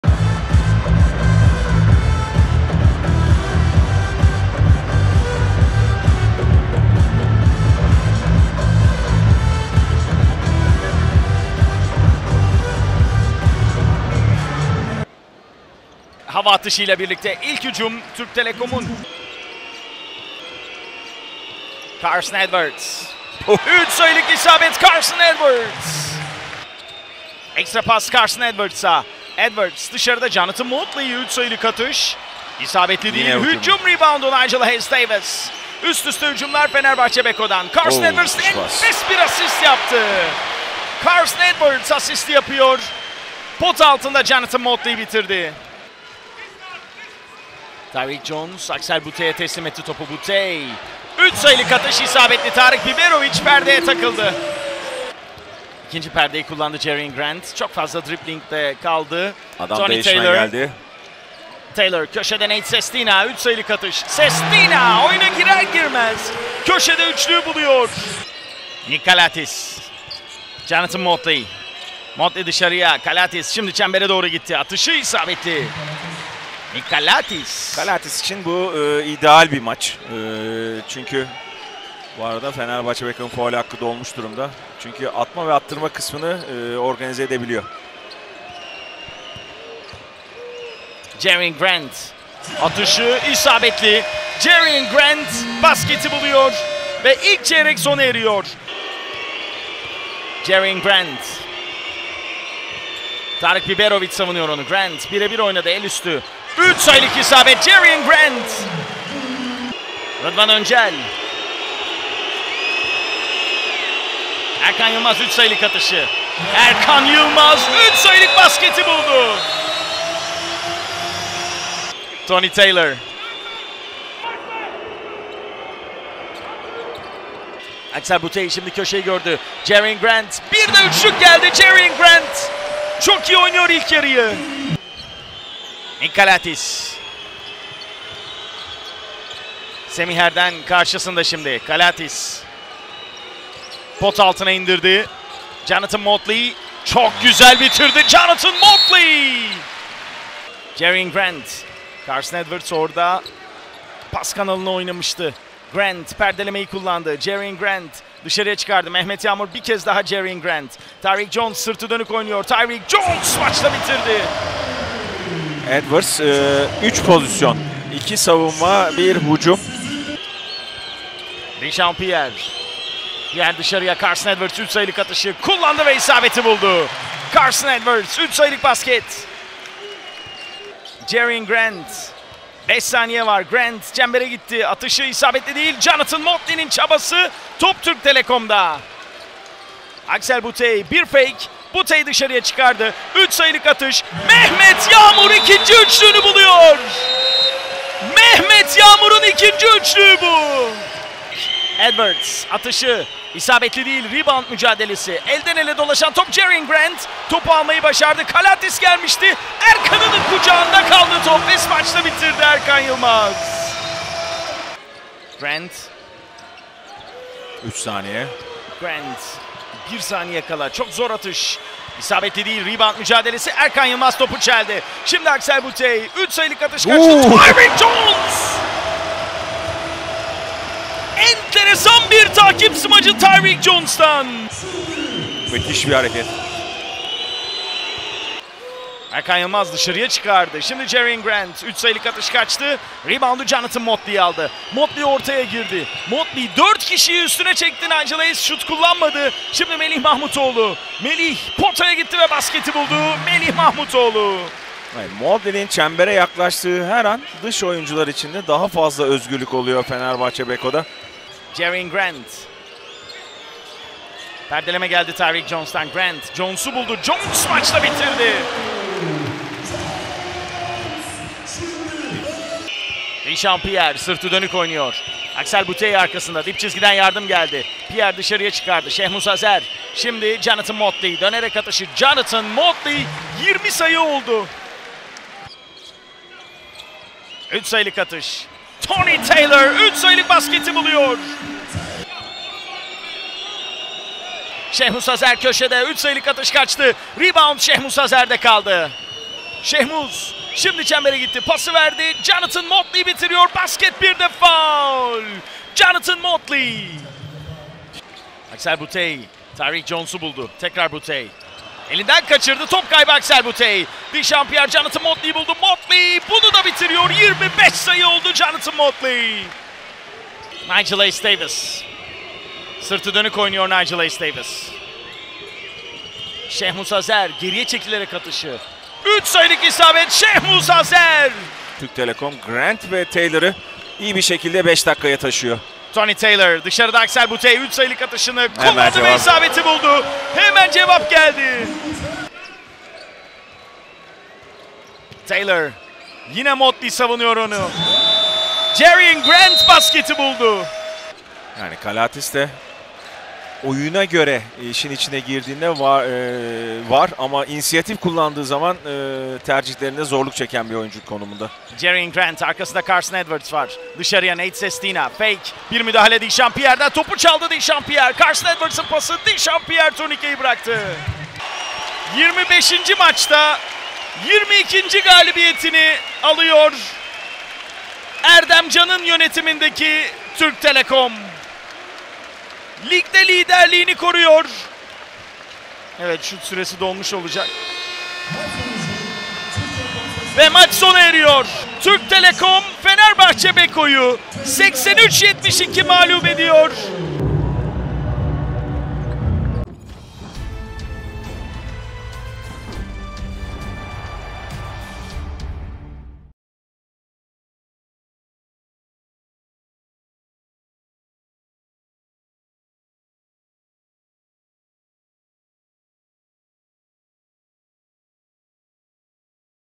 Türk Telekom 83-72 Fenerbahçe Beko | 25. Hafta - Türkiye Sigorta Basketbol Süper Ligi
Türkiye Sigorta Basketbol Süper Ligi'nin 25. haftasında lider Türk Telekom, ikinci sıradaki Fenerbahçe Beko'yu 83-72 mağlup etti.

İlk çeyreği 27-19, devreyi de 45-40 önde tamamlayan Türk Telekom, final periyoduna da 65-57 önde girdi. Son çeyrekte de hata yapmayan Türk Telekom, karşılaşmayı 83-72 kazandı.

Türk Telekom'da 16 sayı kaydeden Jerian Grant galibiyetin mimarı olurken, Fenerbahçe Beko'da Johnathan Motley'in 25 sayısı mağlubiyeti önlemeye yetmedi.

Başkent ekibi bu sonuçla üst üste 11, toplamda da 22. galibiyetini aldı ve Fenerbahçe Beko ile arasındaki farkı 2 galibiyete çıkardı.

Türk Telekom: 83 - Fenerbahçe Beko: 72

Salon: Ankara

Hakemler: Aytuğ Ekti, Can Mavisu, Çağlar Özkan

Türk Telekom: Rıdvan Öncel, Grant 18, Erkan Yılmaz 9, Bouteille 14, Jones 13, Mehmet Yağmur 6, Semih Erden, Berk Demir, Taylor 13, Sestina 10

Fenerbahçe Beko: Şehmus Hazer 6, Edwards 10, Biberovic 6, Hayes-Davis 10, Motley 25, Calathes 4, Pierre 9, İsmet Akpınar, Melih Mahmutoğlu 2

1. Periyot: 27-19

Devre: 45-40

3. Periyot: 65-57